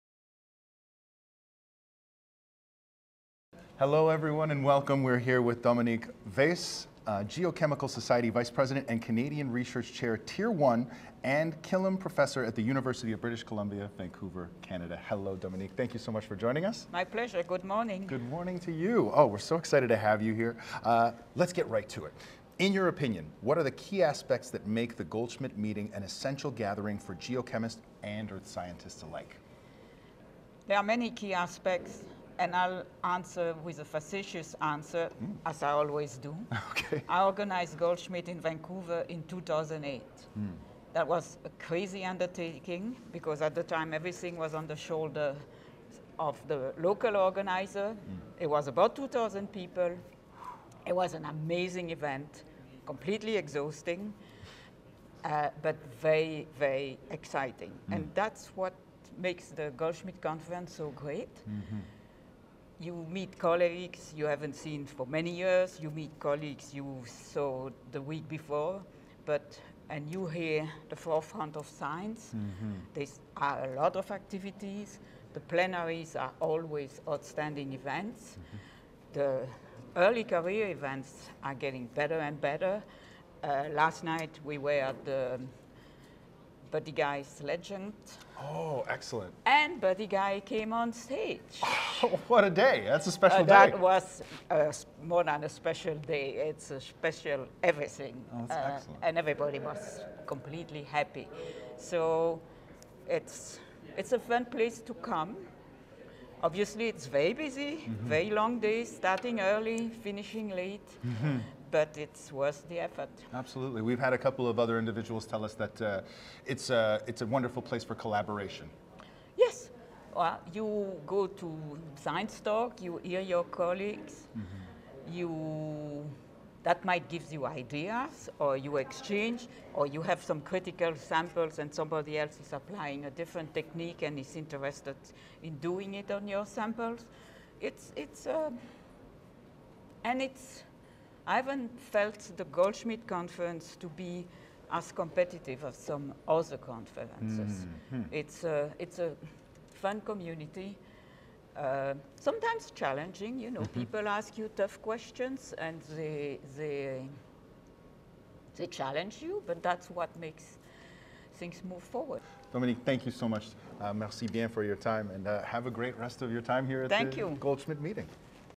Dominique Weis, vice president of the Geochemical Society
Goldschmidt TV introduces Dominique Weis, vice president of the Geochemical Society